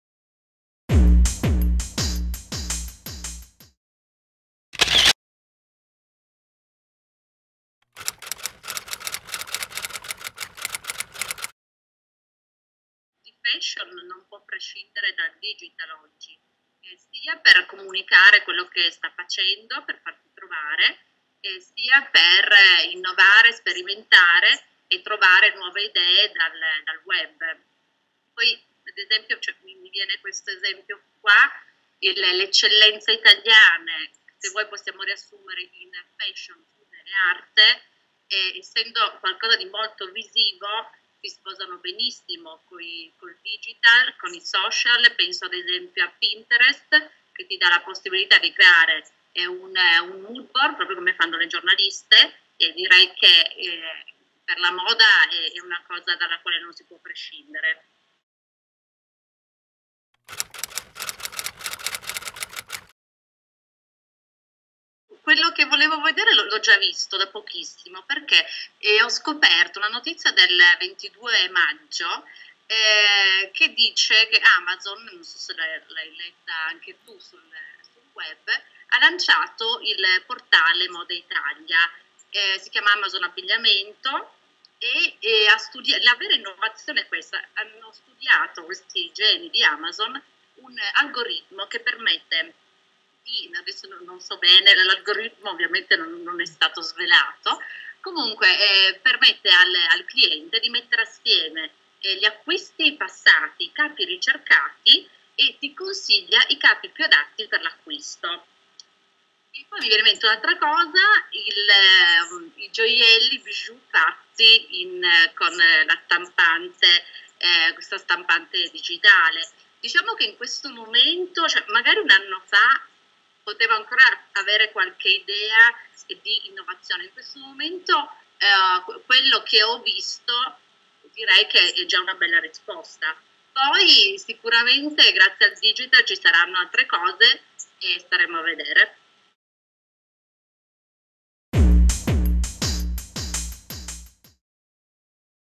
Fashion 2.0. Intervista a Erica Vagliengo
Torino, 31 maggio
Digital Festival 2014

Erica Vagliengo, web journalist & fashion blogger, racconta perché il digital è essenziale per il fashion e quali sono le innovazioni più interessanti che sta apportando in questo settore.
L'intervista raccoglie alcuni spunti emersi durante il panel Fashion: il futuro è oggi, organizzato il 31 maggio 2014 nell'ambito del Digital Festival e moderato da Erica Vagliengo.